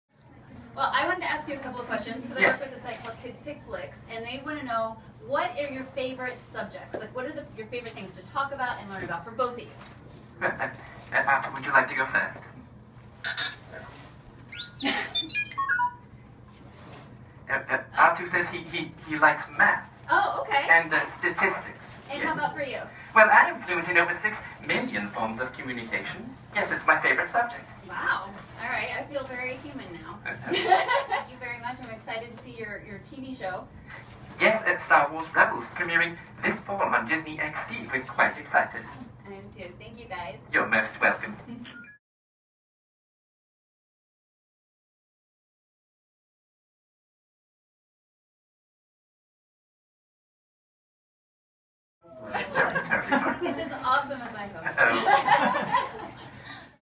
C3PO & R2D2 Interview: Star Wars Rebels
KidsPickFlicks.com's Tara McNamara asks STAR WARS' C3PO and R2D2 about their favorite things and their new show, STAR WARS REBELS.